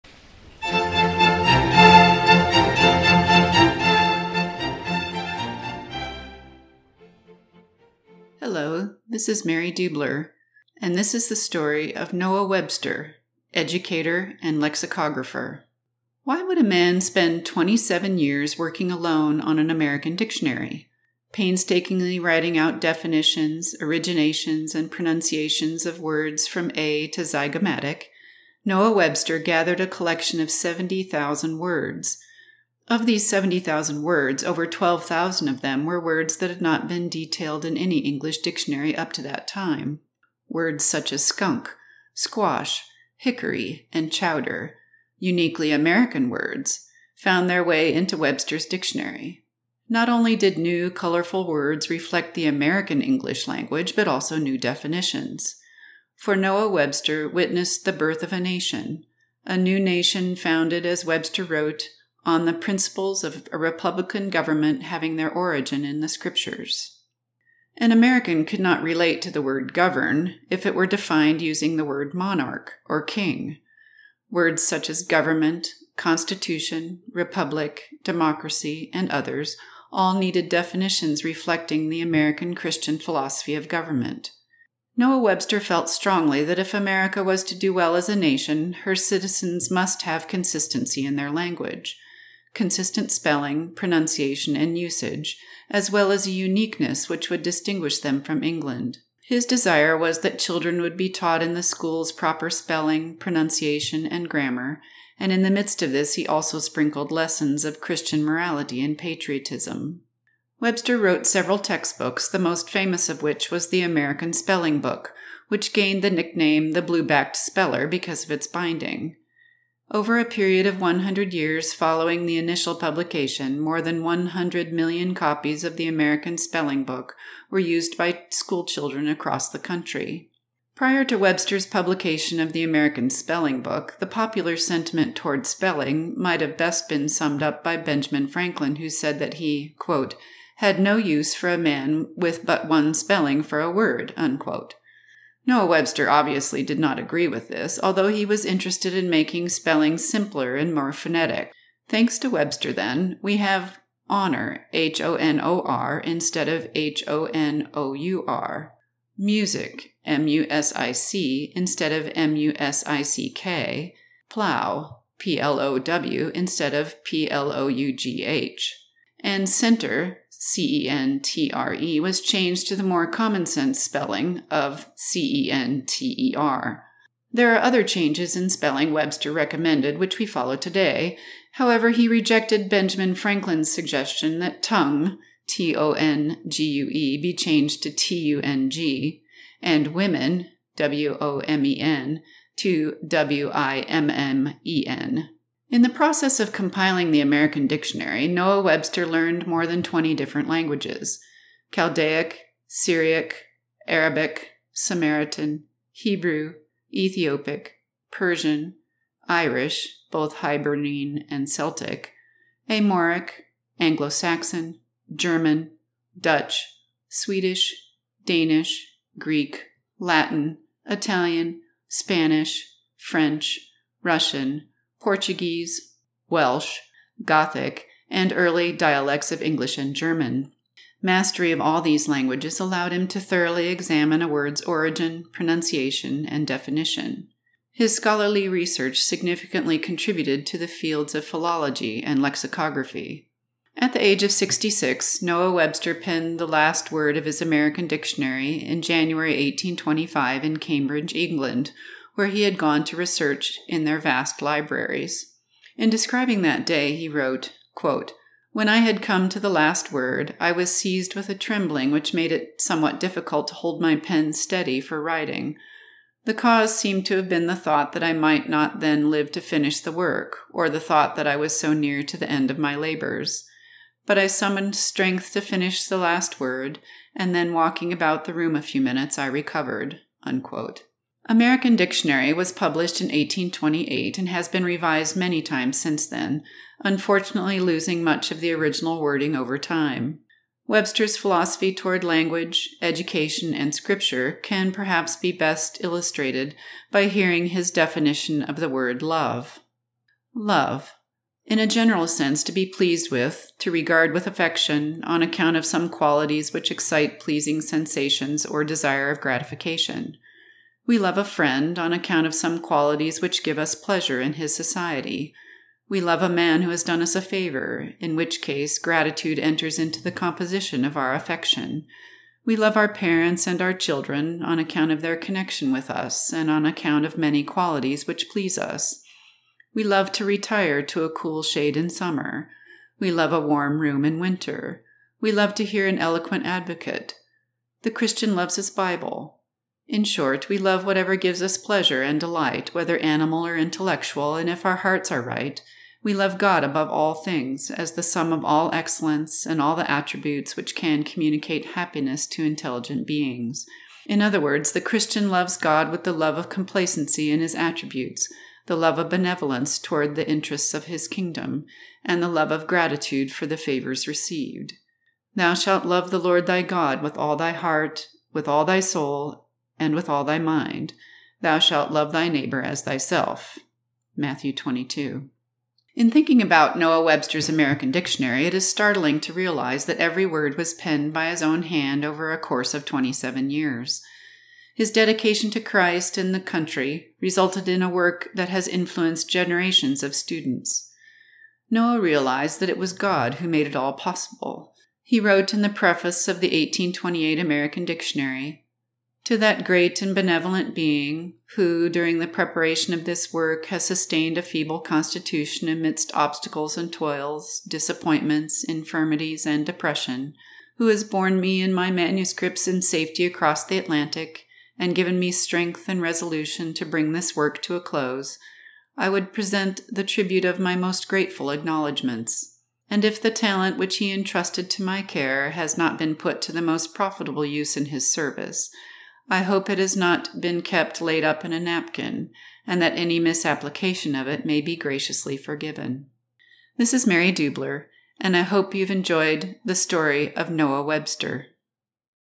Noah Webster, and the first American dictionary.
Meet the man who created the first American dictionary. His work gave shape to the uniquely American spelling, pronunciation, grammar and language usage for generations to come. His deep faith inspires all who learn of his perseverance and dedication to the work which shaped our nation.